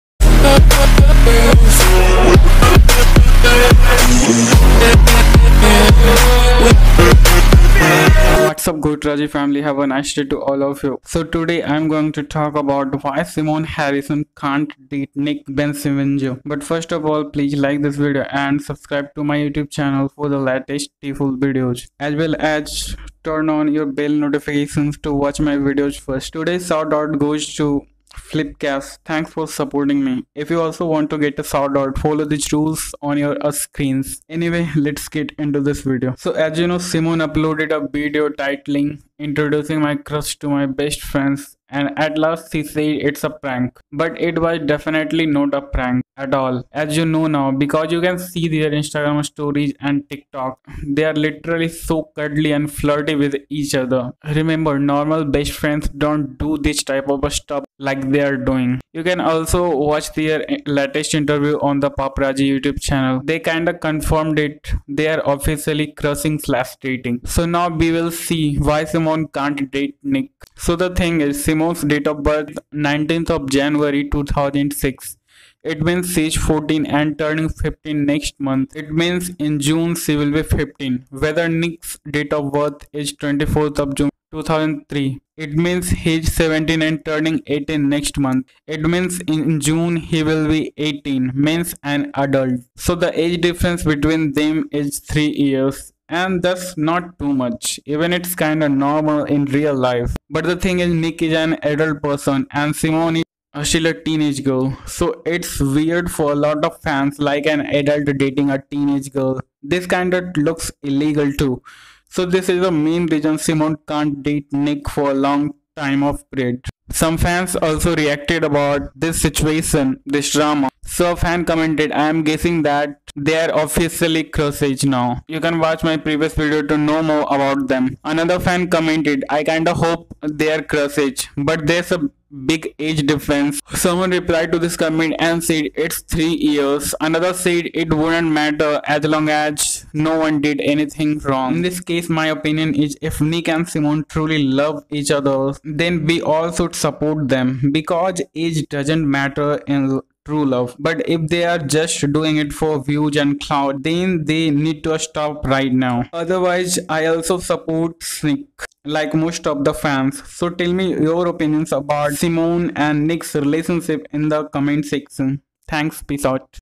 Symonne Harrison and Nick Bencivengo Can't Date
Symonne Harrison has a new crush. His name is Nick Bencivengo. But Symonne can't date Nick for a long time because Nick will be an adult next month whether Symonne is still a teenager so, it's kinda for all of the fans. If they truly love each other then we should support them.

spilling tea every day of the week :)